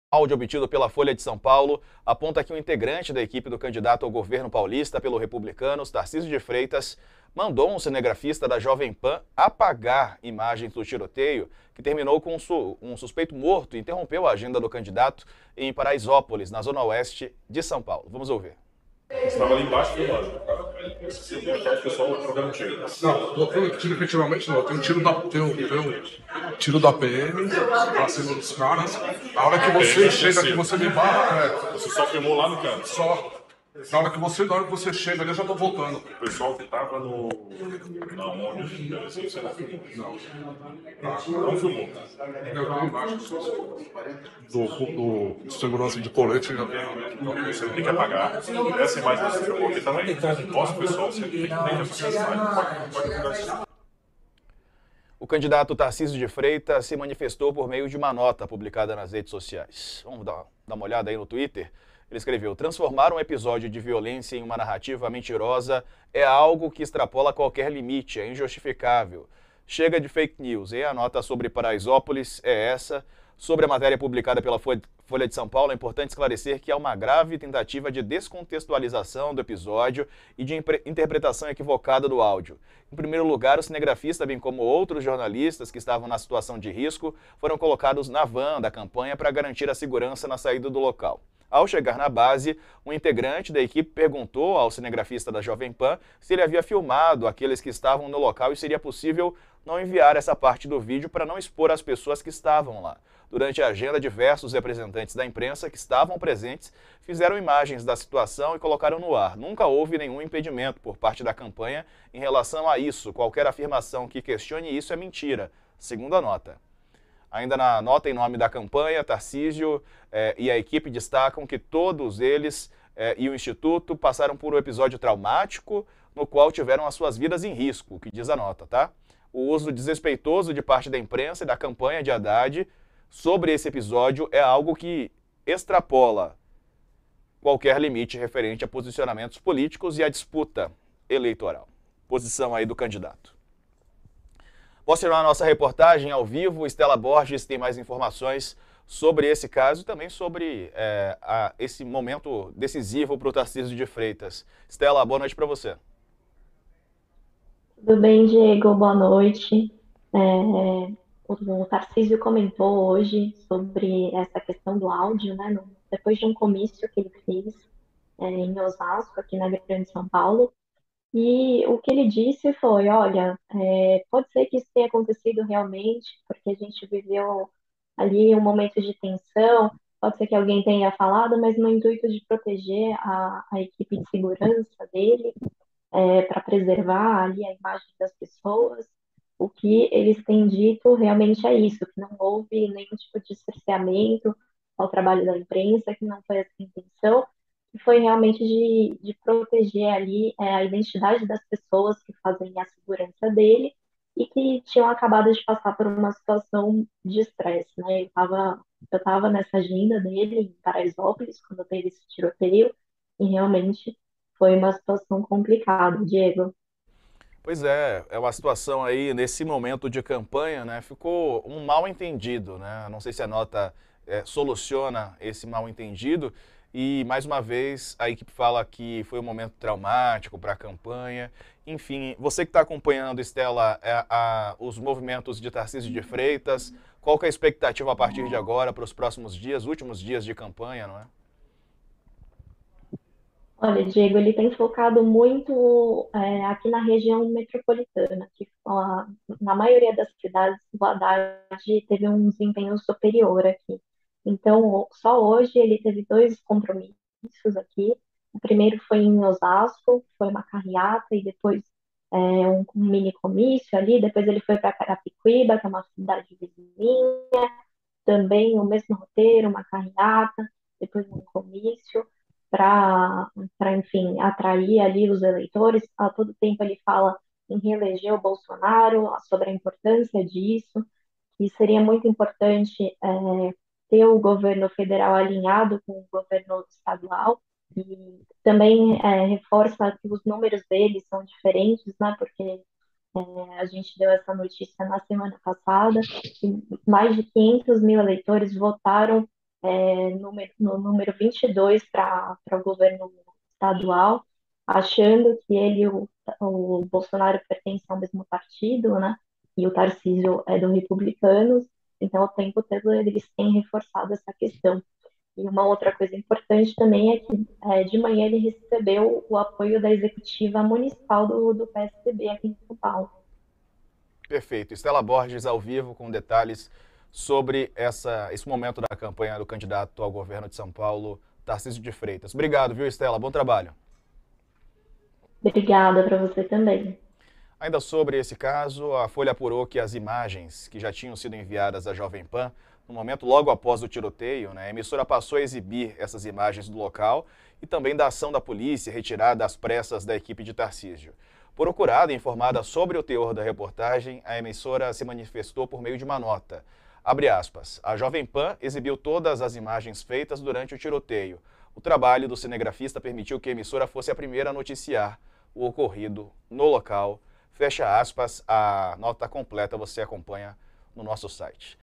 Tarcísio cita 'interpretação equivocada' de áudio sobre Paraisópolis e critica uso político do caso
A equipe de Tarcísio de Freitas (Republicanos), candidato ao governo de São Paulo, confirmou, hoje à tarde, a veracidade do diálogo envolvendo um integrante da campanha, que mandou um cinegrafista da Jovem Pan apagar imagens do tiroteio em meio a um evento com a participação do político em Paraisópolis, zona sul de São Paulo. O episódio ocorreu no dia 17 deste mês e teve um suspeito morto.